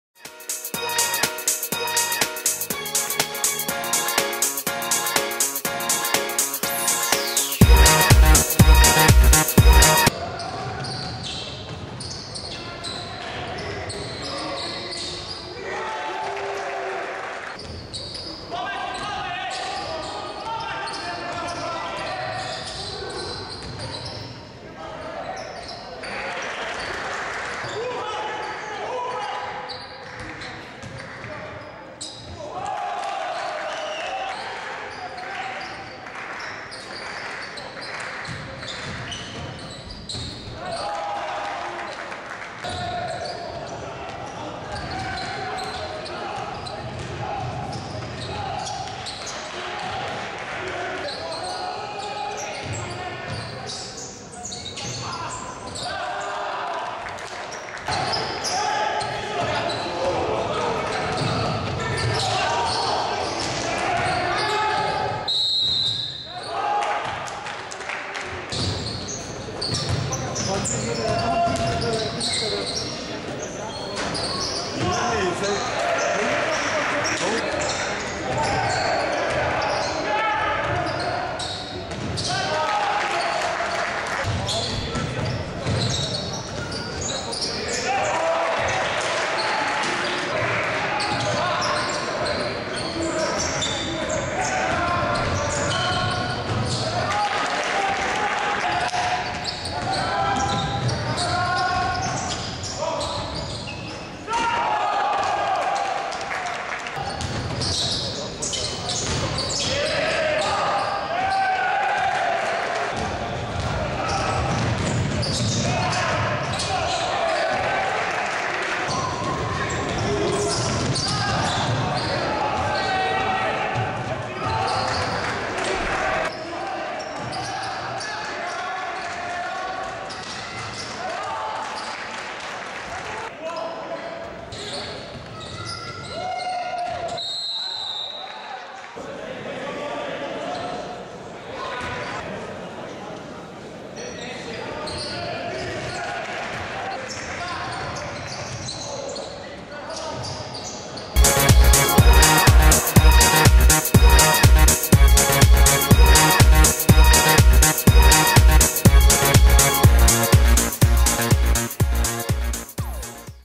Game day 3. Ukraine beats Poerto Riko.
Turk-Telekom Int. Tournament. Ukraine vs. Poerto Riko. BV Vedmedi players: Bezpalyuk, Bolotsenko, Dzyuba, Prokopenko. Head coach: Stanislav Medvedenko